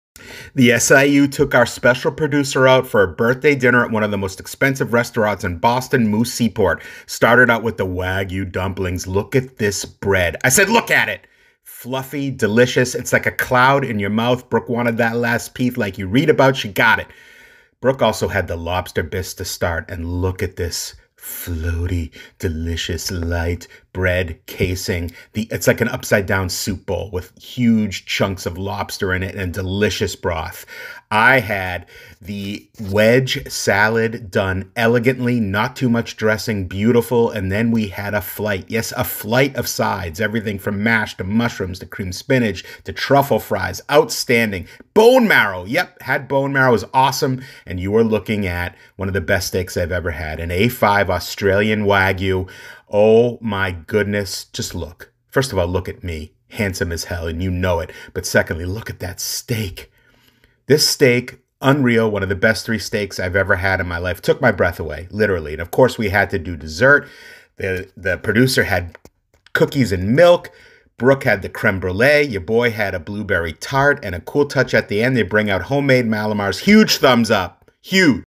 Trying one of the MOST EXPENSIVE RESTAURANTS in Boston! 😮🤑💸💰🤤
To celebrate the Producer's birthday, the SIU family went to one of the MOST EXPENSIVE restauarants in Boston, Mooo Seaport. Does it live up to the high prices and hype? Tune in and find out!

SUBSRIBE TO THE SNACK INVESTIGATION UNIT!!!